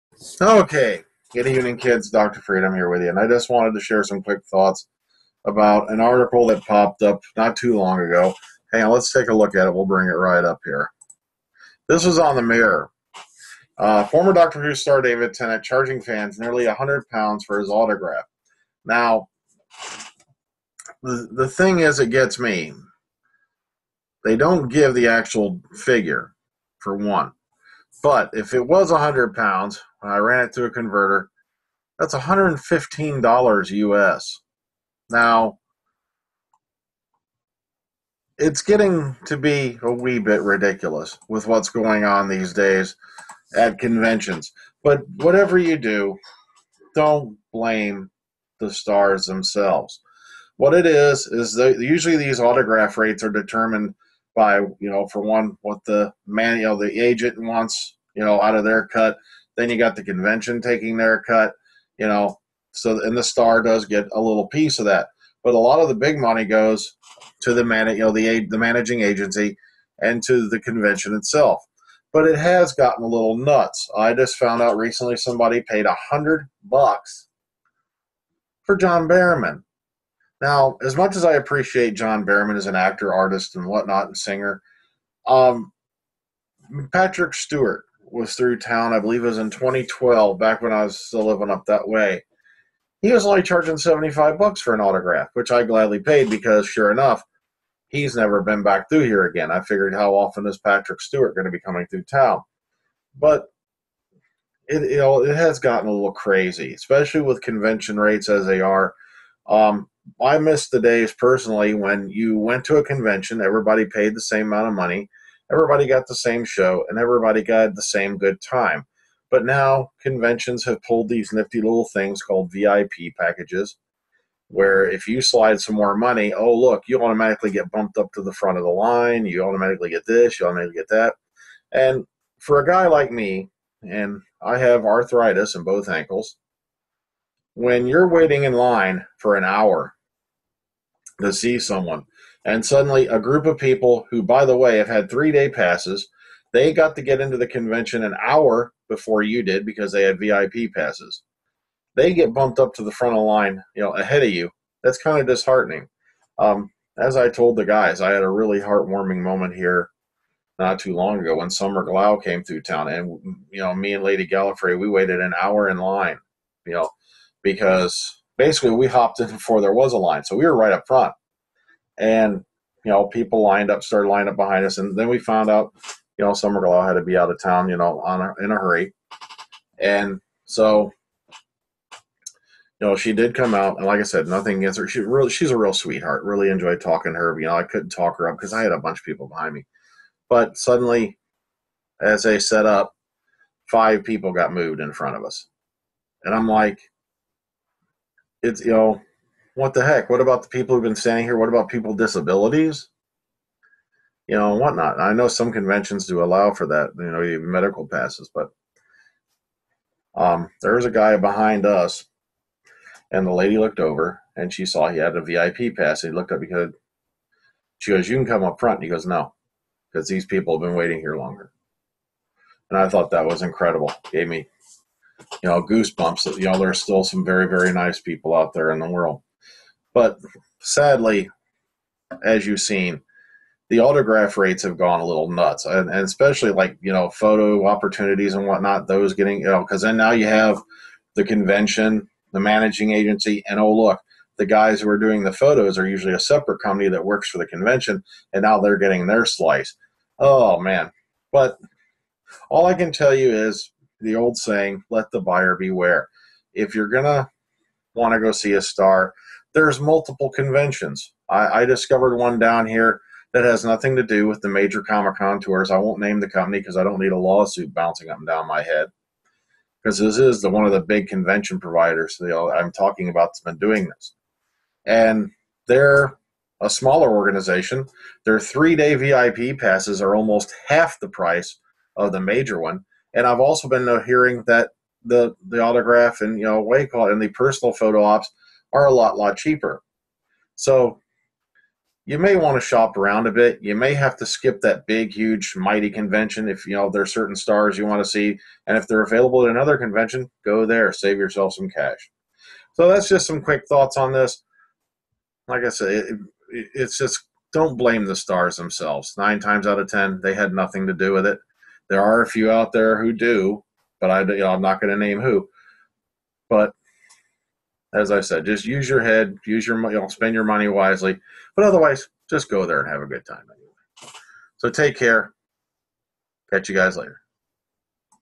FREEDOM UNLEASHED - Some Quick thoughts on Convention Pricing
BE SURE TO LOOK UP DOCTOR FREEDOM ON FACEBOOK!

THE WHOVIANS BLOGSPOT - Program Guide!

DoctorFreedom1

SKYPE:
doctorfreedom1

SOURCES:
Former Doctor Who star David Tennant charging fans nearly £100 for his autograph